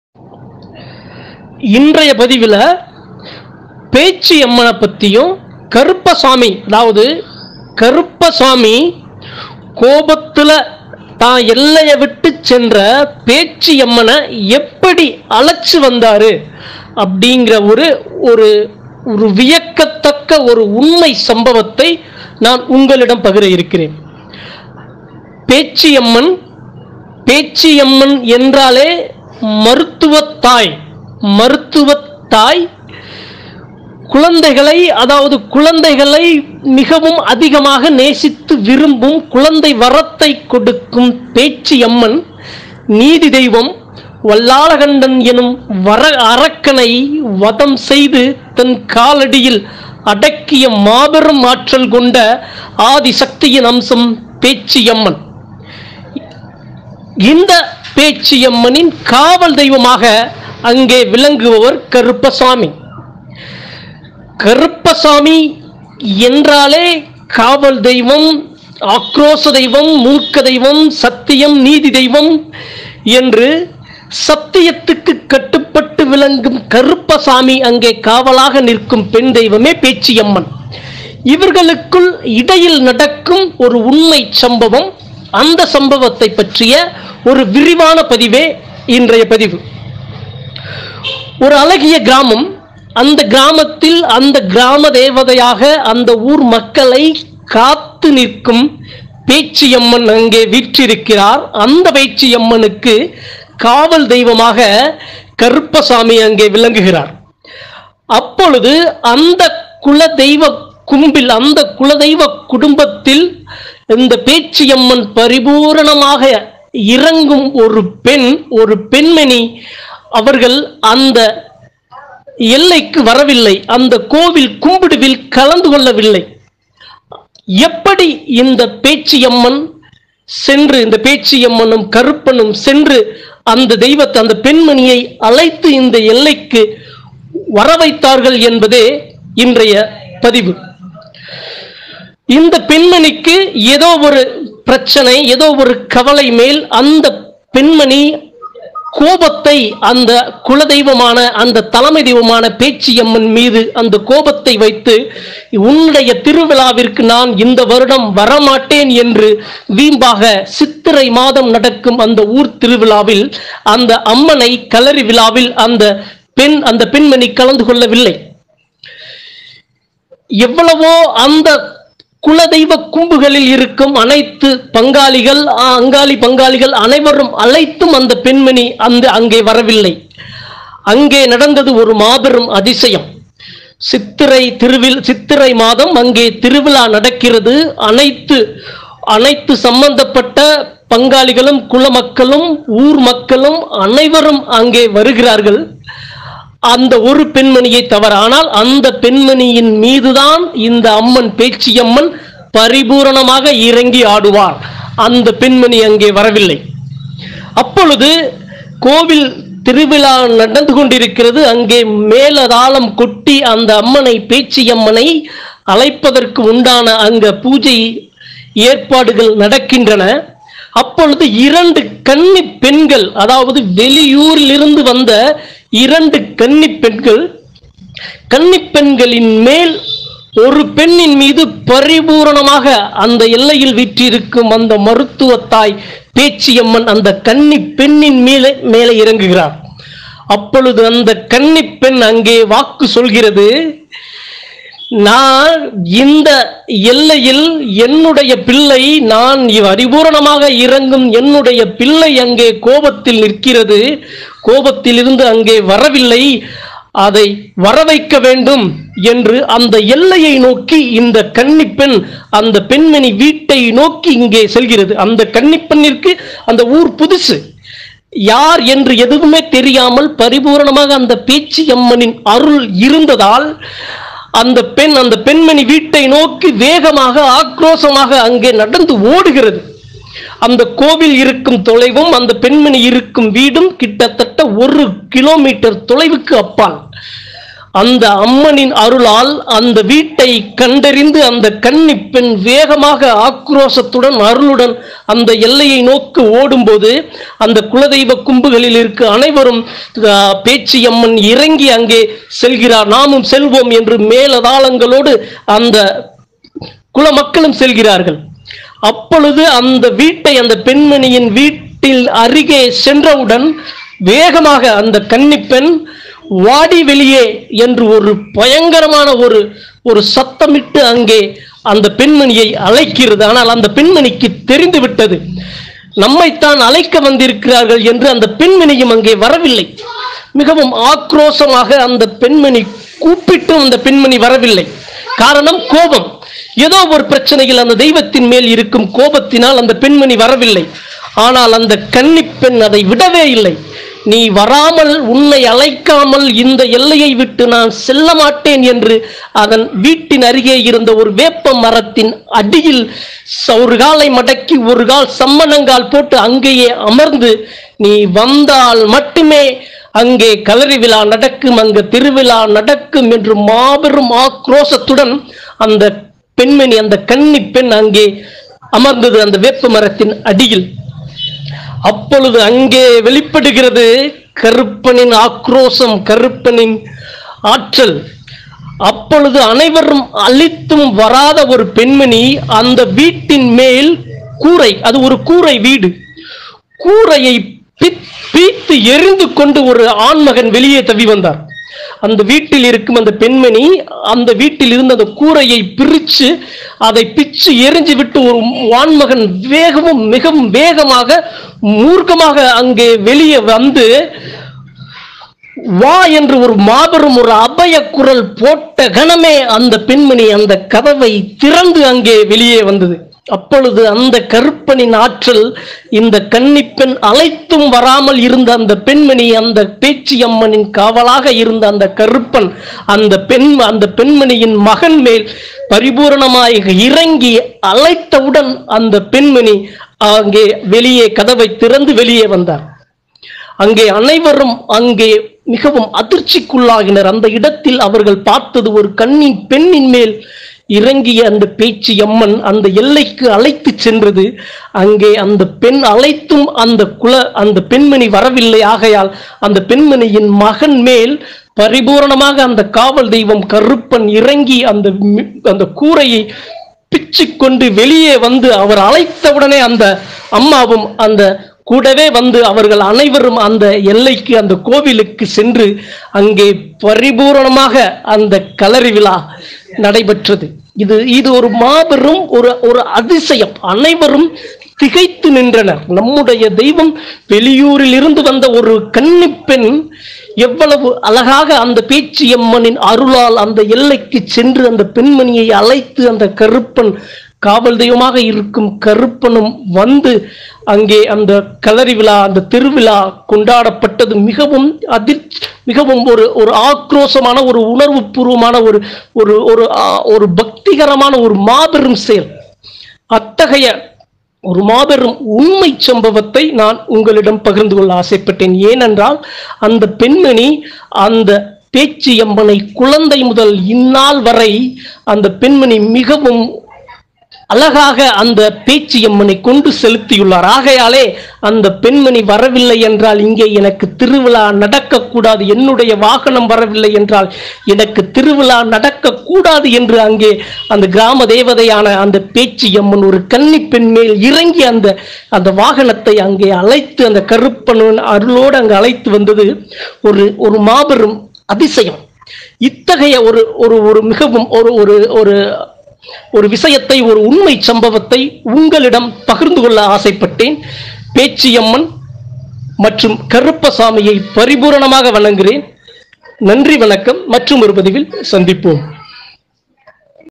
Pechi Amman | கூரையை பிரித்து பேச்சி அம்மனை அழைத்து வந்த கருப்பன் | Karuppasamy God
Udukkai | உடுக்கையில் உயிர் வாழும் 21 தெய்வம் | உண்மை சம்பவம்